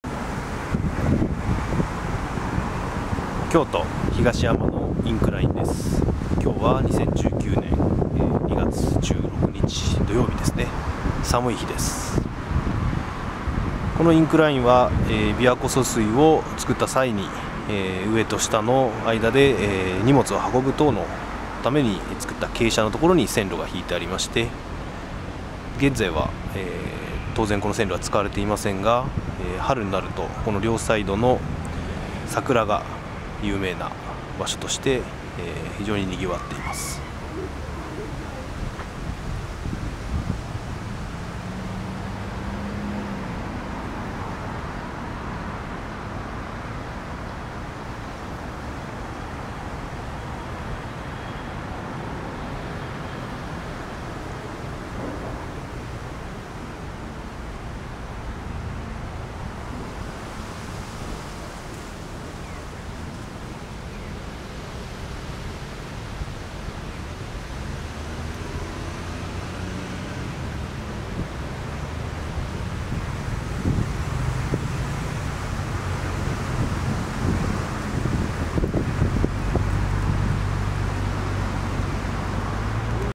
【未来への1分】2019/02/16 蹴上インクライン 京都府京都市東山区 X-T3 4K60P
【未来への1分】＝未来の人が見たときに分かりやすいように「今・この時」を解説。
録画開始時刻：2019/02/16 15:10, 平成31年2月16日
住所：京都府京都市 東山区 東小物座町３３９
撮影方向：蹴上インクライン (南南東)
撮影機材：Fujifilm X-T3 + XF10-24mmF4 R OIS + Tripod : 4K60P(H.265, 200Mbps)
ライセンス：クリエイティブコモンズ（将来の人類の為に今を高画質で残す）